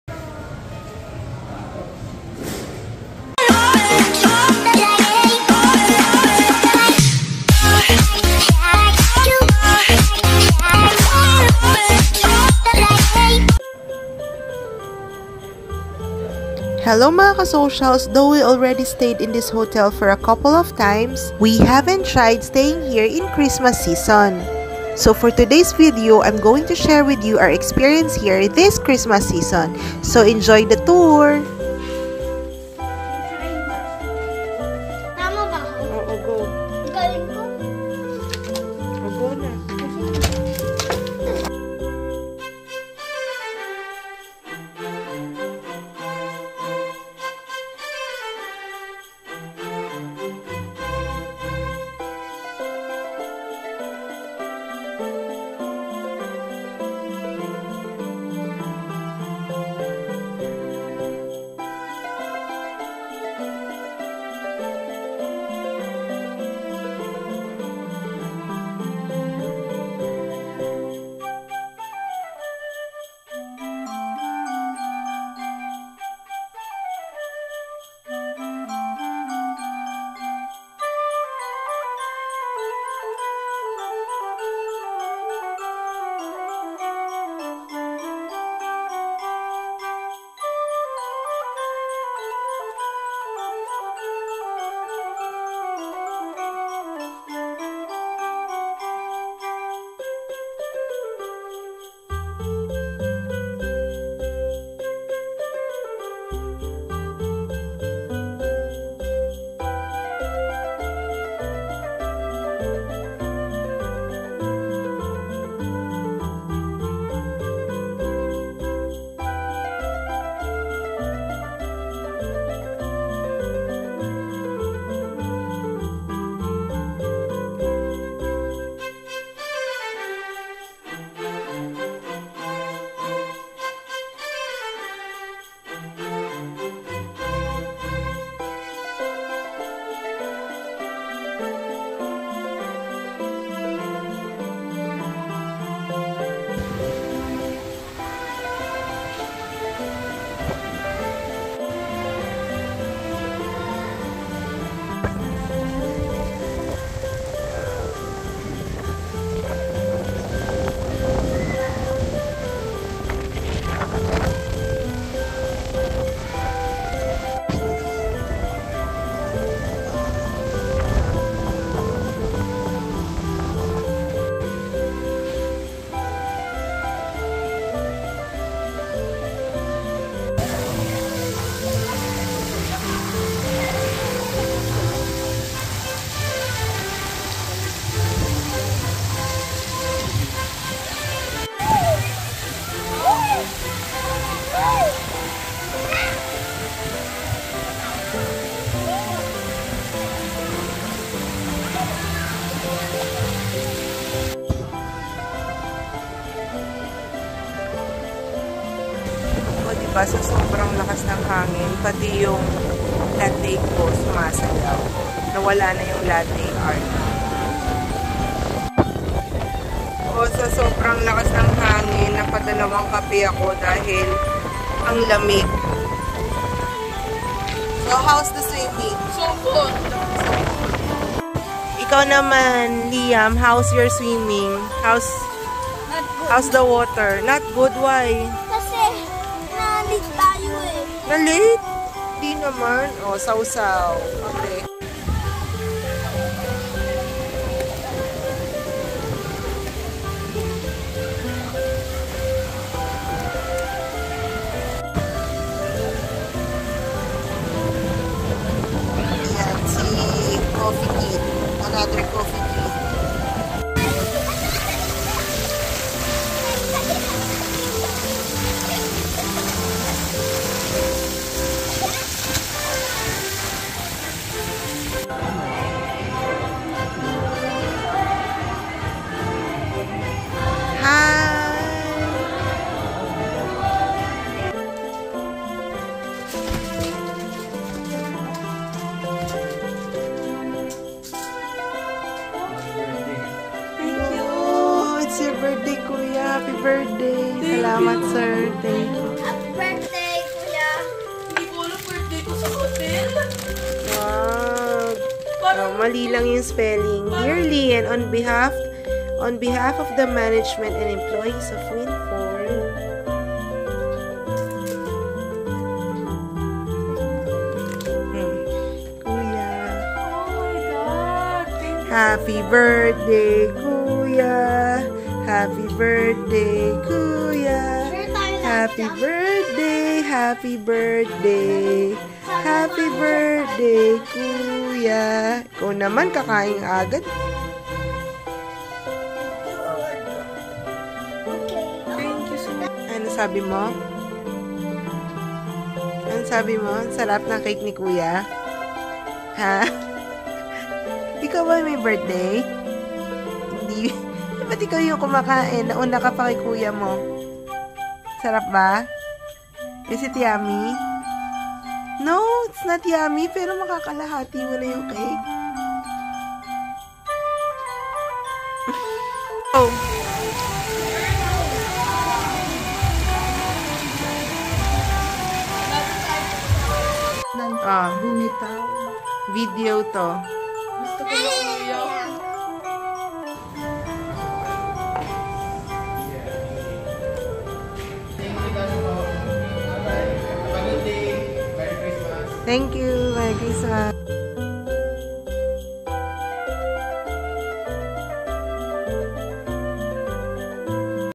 WINFORD HOTEL RESORT AND CASINO | CHRISTMAS SEASON | STAYCATION | CHESKApade
Date published: January 10, 2023

We usually stay in this hotel because first, it's not too far compared to other hotels, second, the kids really like the heated pool thou when they get out of the pool, the wind is so cold and third because of breakfast.

Watch the video until the end and see if you can
consider this hotel on your next Holiday staycation.